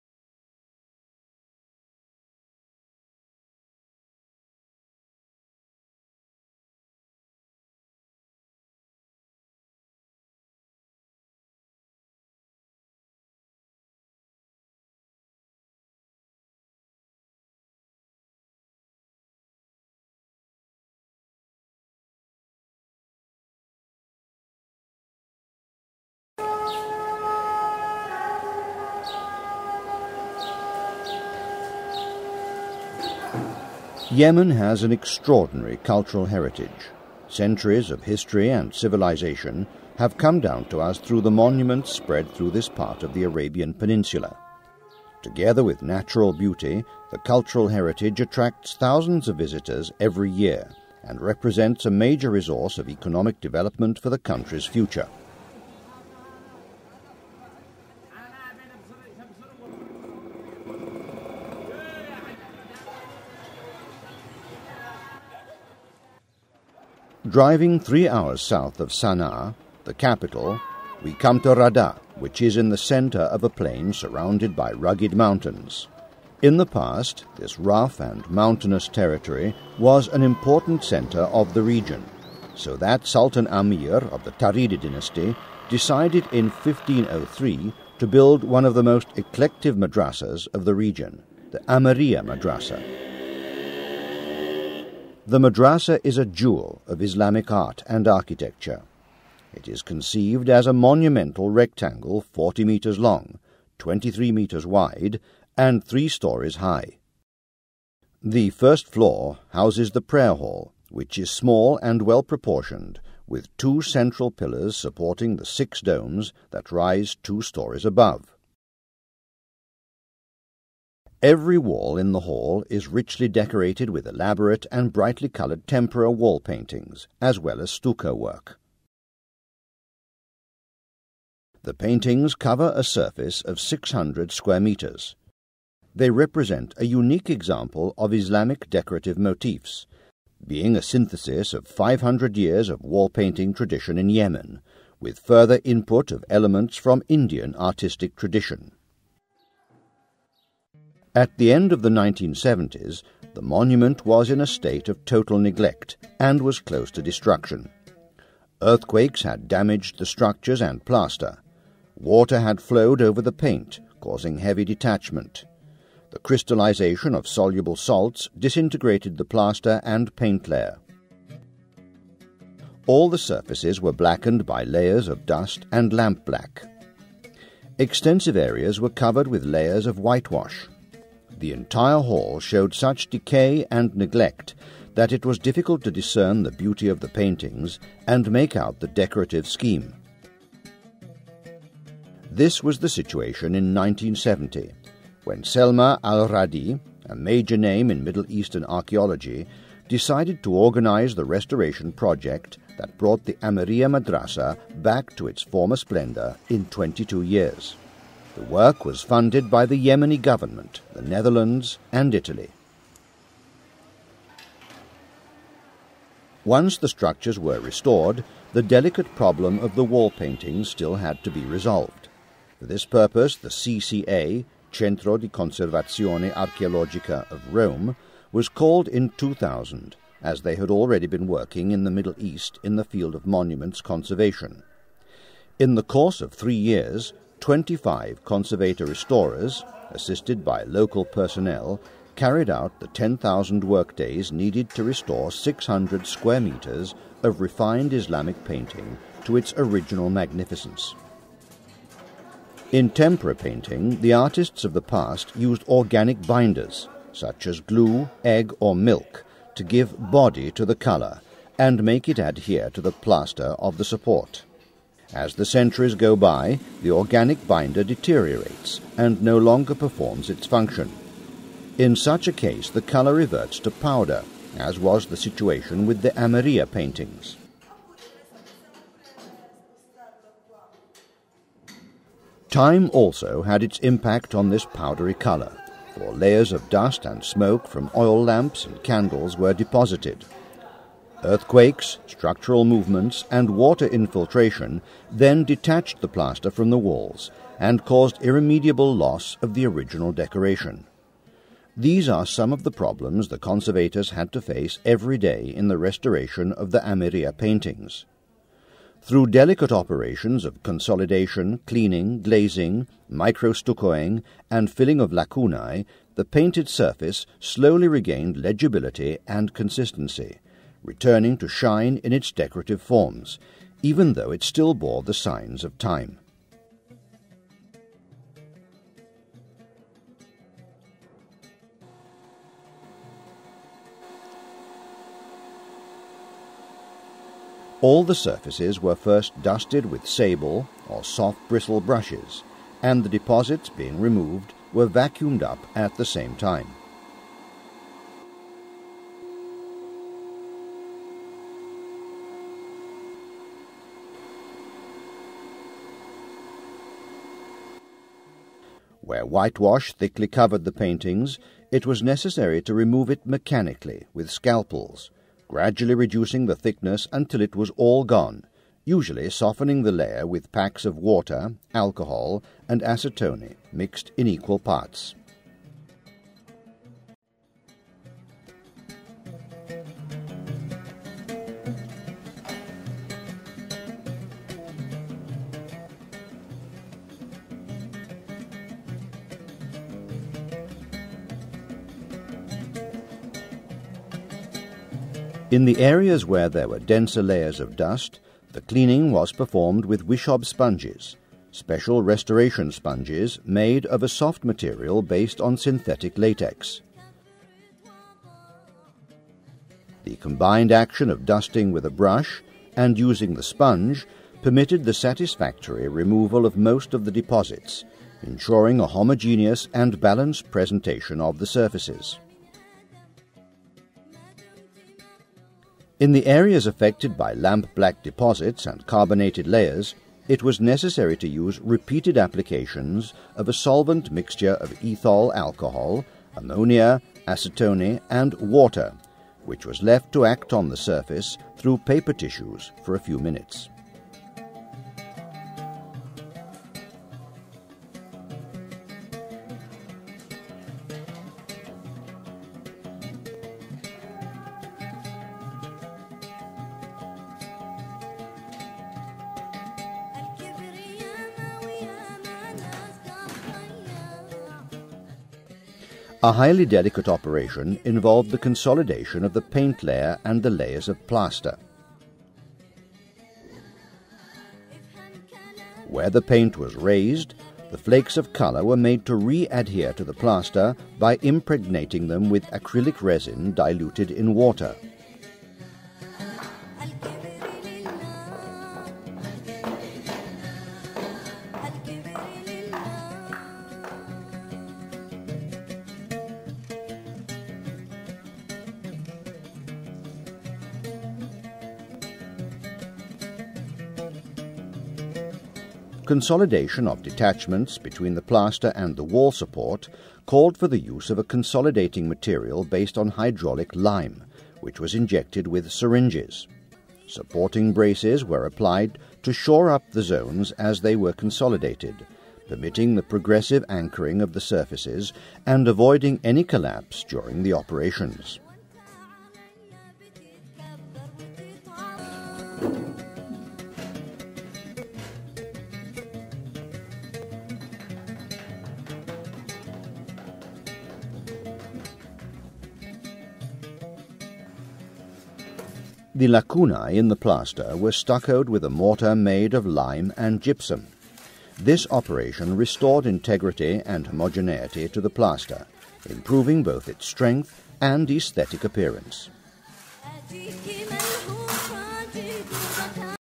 Amiriya Madrasa. Part 1. The Conservation of the Mural Paintings
The video describes the restoration of the mural paintings of the Amiriya Madrasa (1504), Radà, Yemen. The intervention was carried out by the Centro di Conservazione Archeologica of Rome (CCA), directed by Roberto Nardi. The project received the Aga Khan Award for Architecture in 2007.